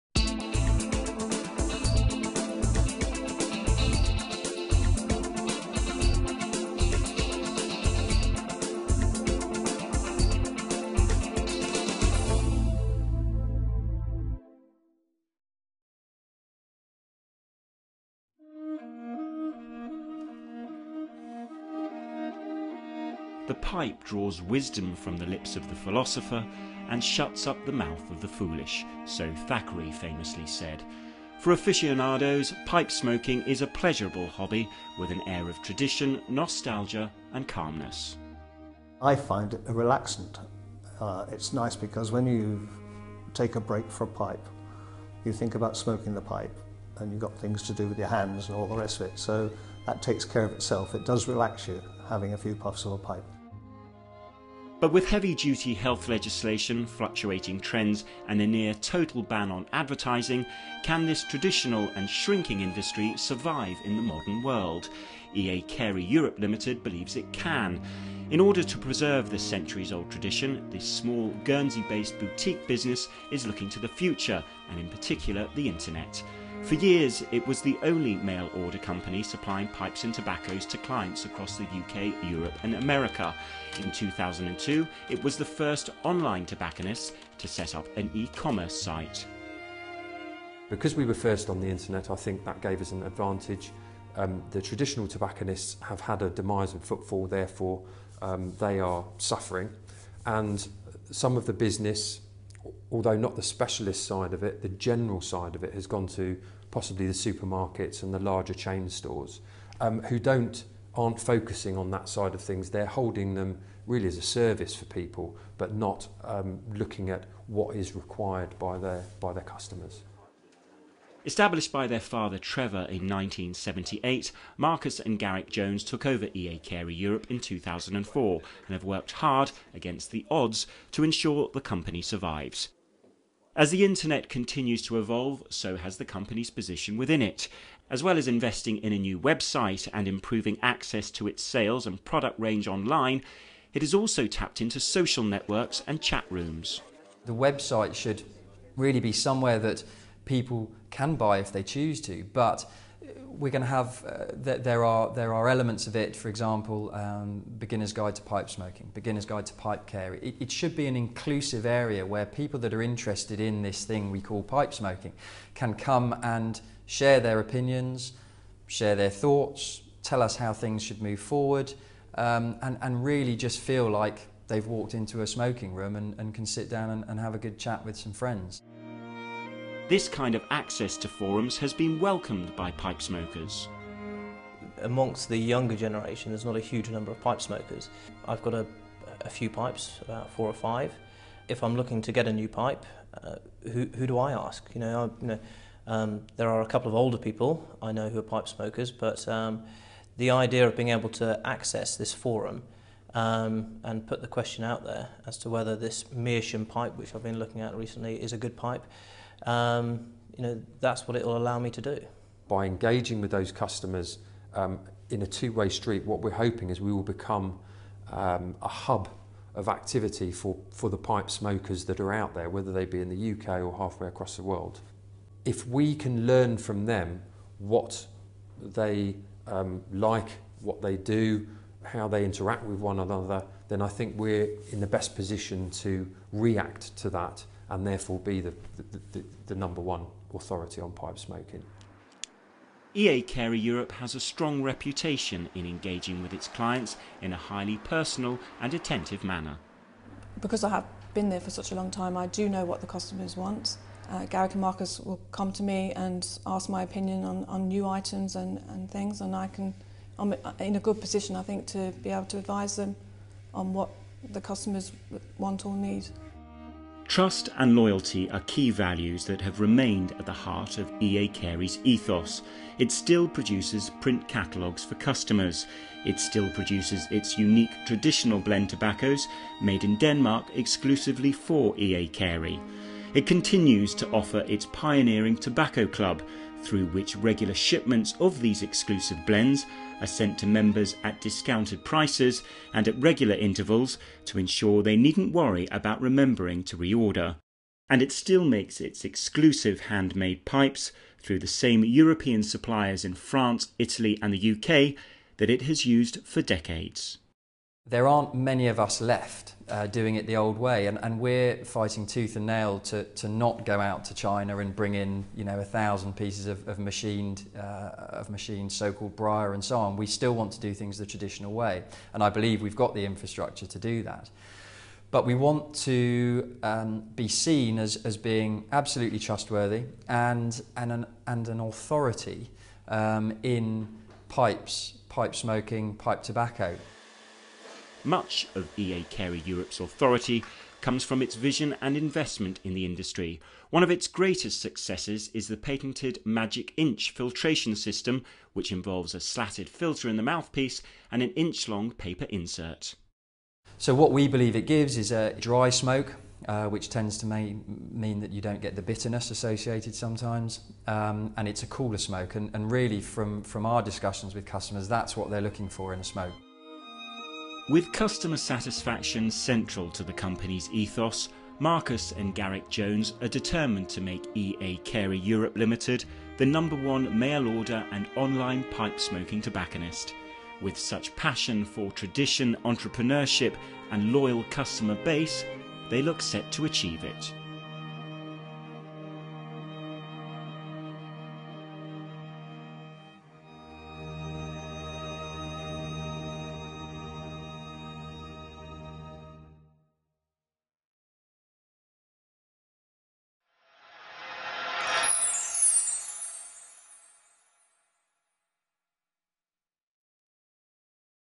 EA Carey (Europe) Ltd - Is there a future in pipe smoking?
With ever tightening health regulations, changes in trends and strict limitations on advertising the industry is fighting to survive.
Against the odds, one Guernsey-based family run pipe and tobacco suppliers is challenging preconceptions to secure a future.
With energy and passion, entrepreneurs Marcus and Garrick Jones explain to the Telegraph Business Club how they are revitalising their family business, EA Carey (Europe) Ltd, by tapping into e-commerce and online social media whilst staying true to traditions that have helped retain long standing clients.
The brothers also give an insight into EA Carey's USPs, which include a discounted tobacco ordering club, handcrafted pipes, exclusive brands and a unique patented filtration system.